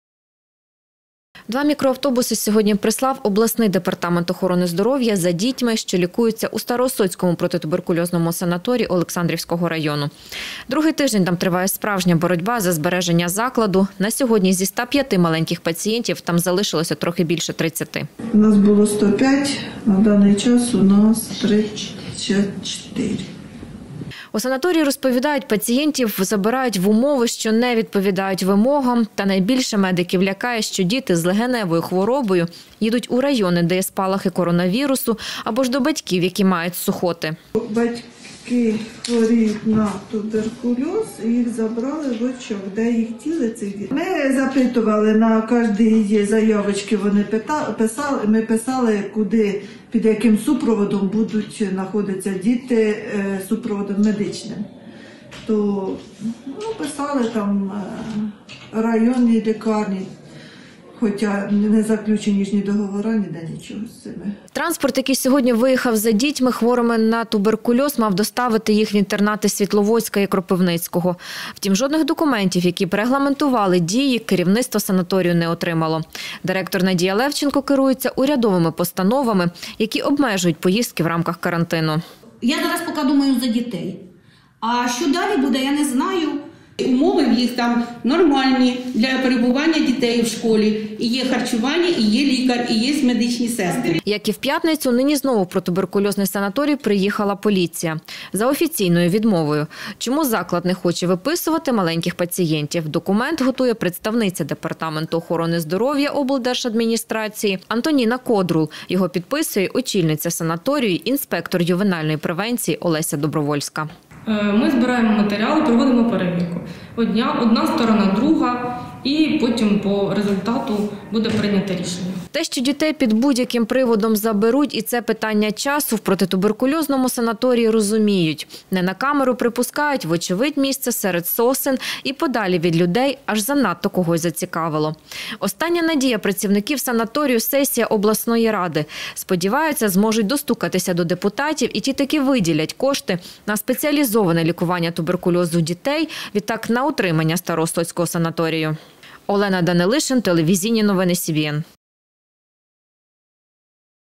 Зі Староосотського протитуберкульозного санаторію вивозять дітей | телеканал Вітер
Кілька мікроавтобусів сьогодні прислав обласний департамент охорони здоровʼя за дітьми, що лікуються у Староосотському протитуберкульозному санаторії Олександрівського району. Другий тиждень там триває справжня боротьба за збереження закладу. На сьогодні зі ста пʼяти маленьких пацієнтів там залишилося трохи більше тридцяти. У санаторії розповідають, пацієнтів забирають в умови, що не відповідають вимогам. Та найбільше медиків лякає, що діти з легеневою хворобою їдуть у райони, де є спалахи коронавірусу, або ж до батьків, які самі мають сухоти.